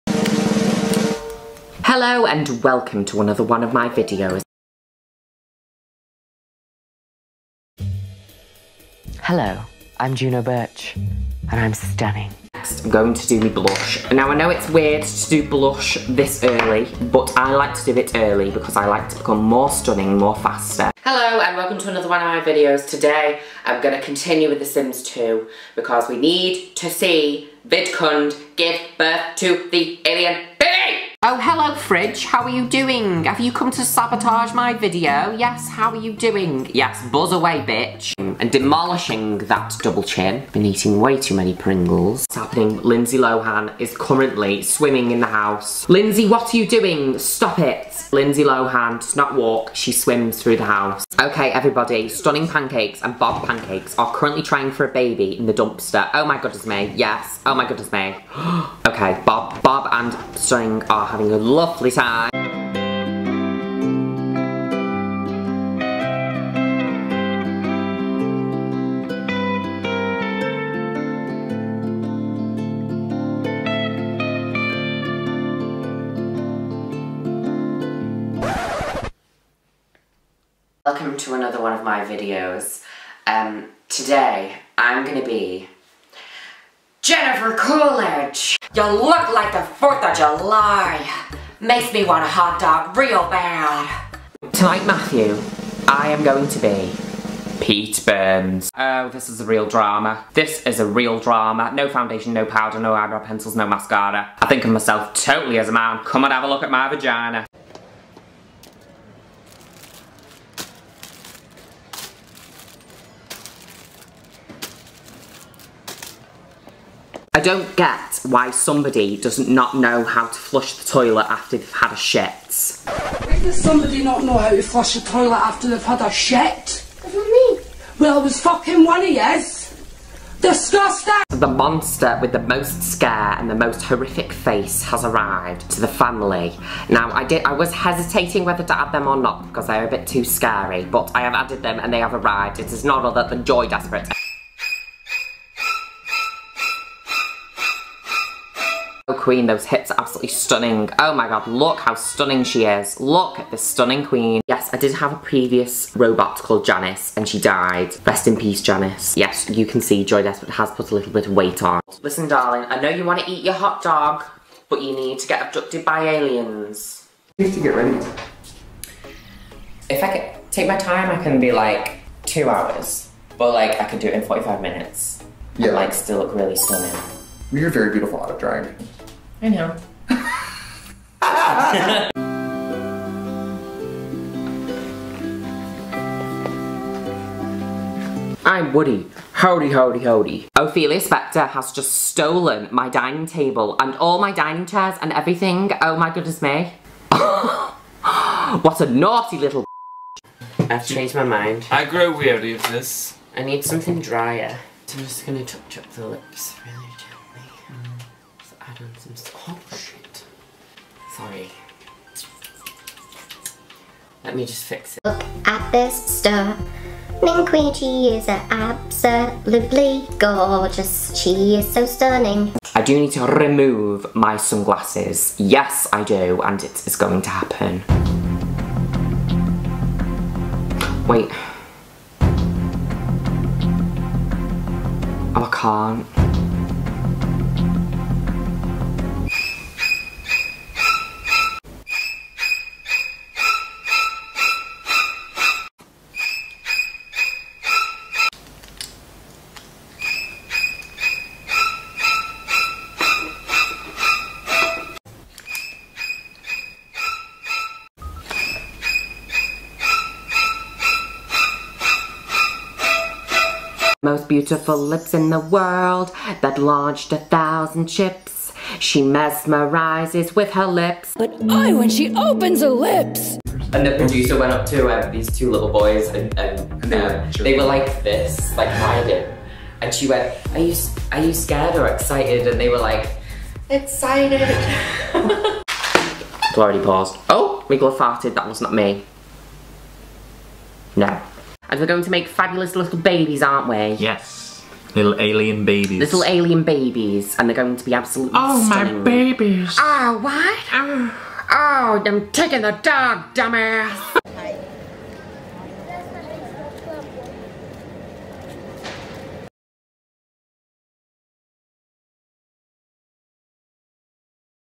Juno Birch being STUNNING for five minutes straight
Every video shown belongs to the ICONIC Juno Birch! Enjoy!

Yes, a compilation of the alien queen herself, Juno Birch. 🤩